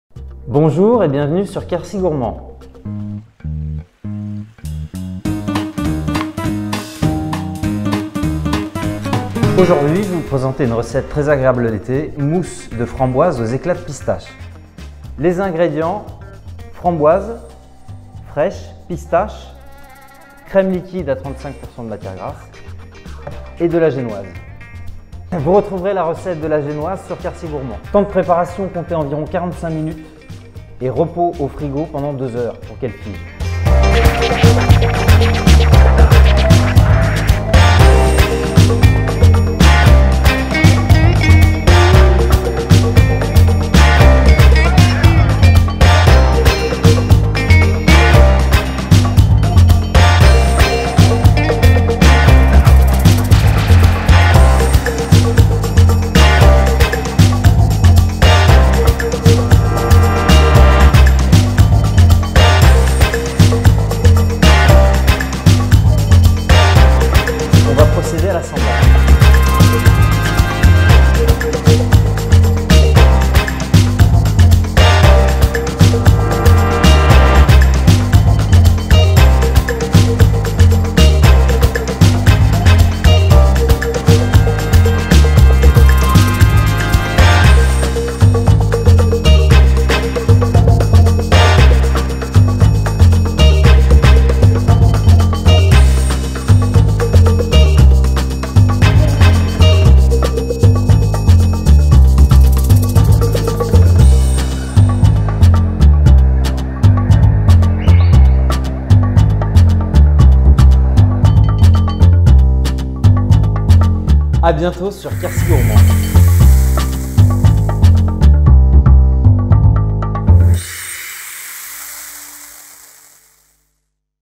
Recette : Mousse framboise, éclats pistaches - quercygourmand.tv
Par Ludovic SOUPIROT Restaurant "La Récréation"
46250 Les Arques
Membre des "Bonnes Tables du Lot"

INGREDIENTS :
- Framboise fraiche 200 ml
- Eclats de pistaches
- Crème liquide 35% 200 ml
- 2 ronds de génoise

repos au réfrigérateur : 2 heures minimum